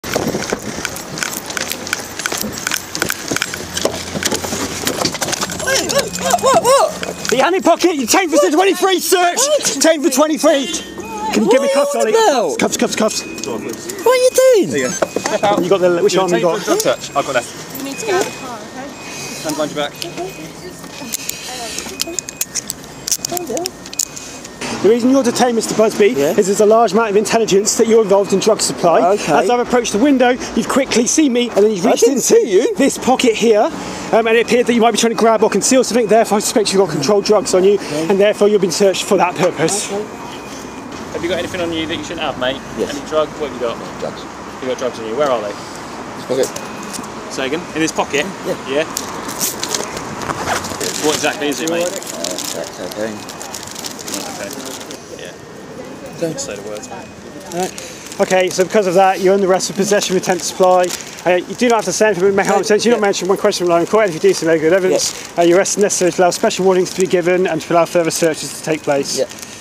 Got anything you shouldn’t have? Yes, DRUGS
A man who boasted he was the ‘best drug dealer about’ has been jailed for more than five years.
Police were on patrol in Cambridge on 1 December when they spotted Robert Busby parked on the pavement on Cherry Hinton Road.
With lots of intelligence around him dealing drugs, they went to question him but he attempted to drive off.
The 54-year-old, of no fixed address, was then searched and found to be in possession of crack cocaine and heroin with an estimated street value of £1,390.